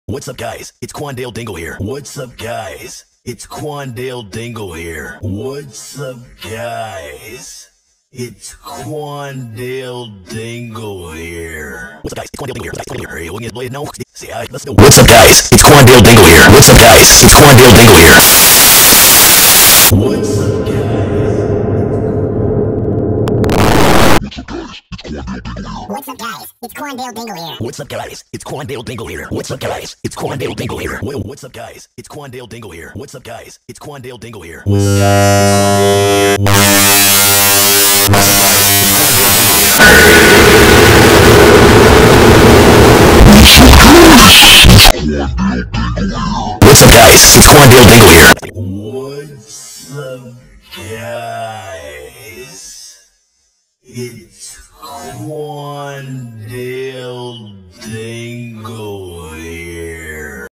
25 Quandale Dingle Sound Variations in 60 Seconds
hey quandale dingle here, i have recently been made into 25 different versions of the same sound